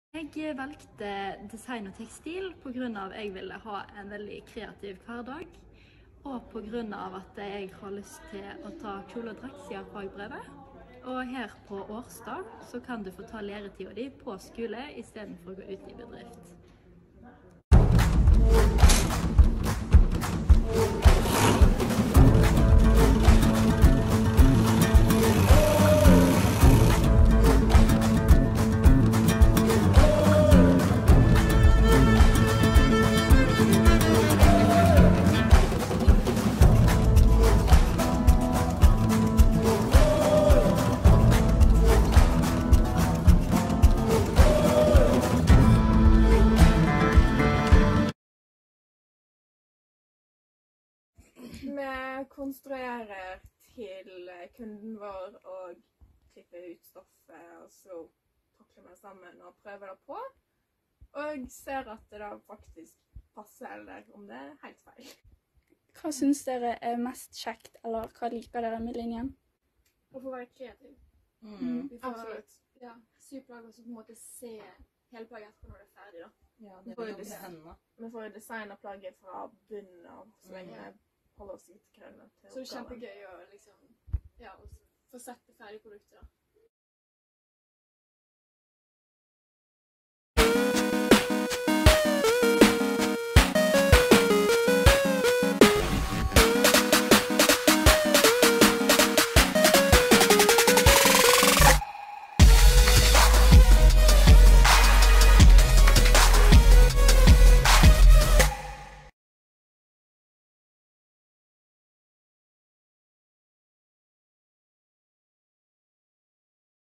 Håndverk, design og produktutvikling med Kjole og draktsyerfag
Et møte med elevene våre på VG2 og VG3 og en liten titt på arbeidet deres og hverdagen.